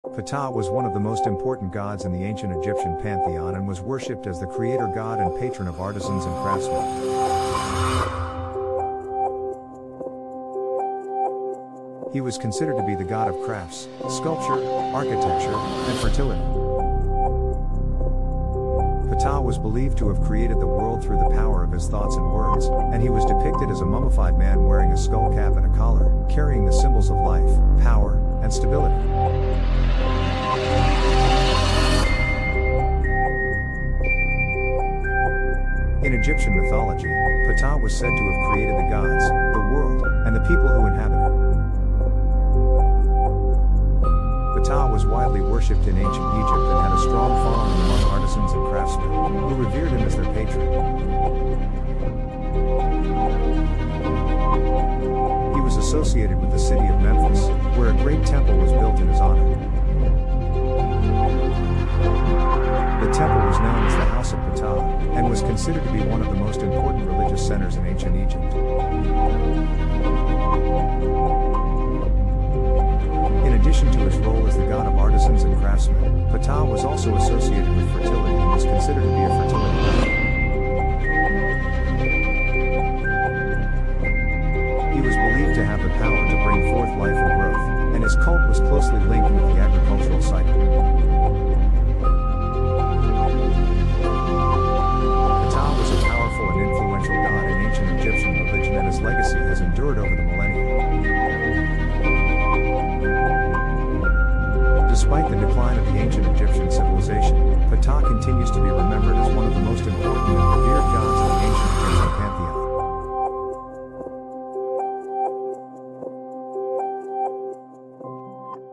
Ptah: The God of Creation and Renewal in Ancient Egyptian Mythology
Ptah was one of the most important gods in the ancient Egyptian pantheon and was worshipped as the creator god and patron of artisans and craftsmen. He was considered to be the god of crafts, sculpture, architecture, and fertility.

Ptah was believed to have created the world through the power of his thoughts and words, and he was depicted as a mummified man wearing a skullcap and a collar, carrying the symbols of life, power, and stability. In Egyptian mythology, Ptah was said to have created the gods, the world, and the people who inhabit it.

Ptah was widely worshipped in ancient Egypt and had a strong following among artisans and craftsmen, who revered him as their patron. He was associated with the city of Memphis, where a great temple was built in his honor. The temple was known as the House of Ptah, and was considered to be one of the most important religious centers in ancient Egypt.

In addition to his role as the god of artisans and craftsmen, Ptah was also associated with fertility and was considered to be a fertility god. He was believed to have the power to bring forth life and growth, and his cult was closely linked with the agricultural cycle.

Ptah was a powerful and influential god in ancient Egyptian religion and his legacy has endured over the millennia. Despite the decline of the ancient Egyptian civilization, Ptah continues to be remembered as one of the most important and revered gods of the ancient Egyptian pantheon.